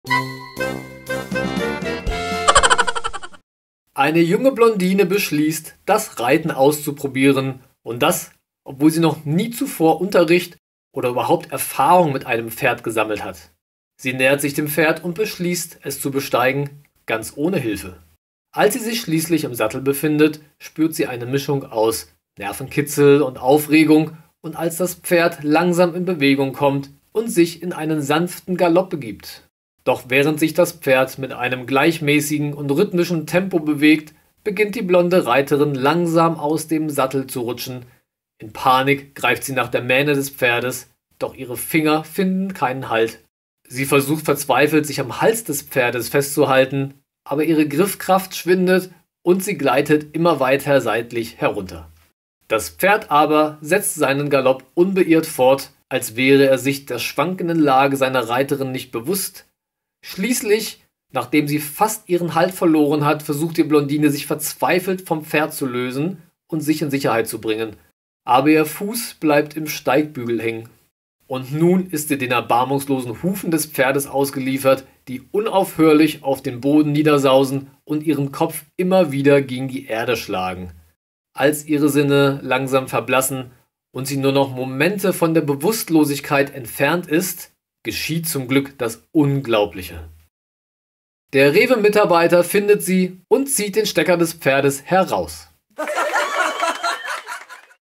🤣 BESTER WITZ DES TAGES! - Eine Blondine beschließt das Reiten zu testen... | Täglich Witzige Videos
👉 Möchtest auch du Netflix, Spotify, Disney+ und weitere Services für einen Bruchteil des Preises bekommst?

🤣 Die lustigsten Witze
😁 Schwarzer Humor
🎤 Mein Mikrofon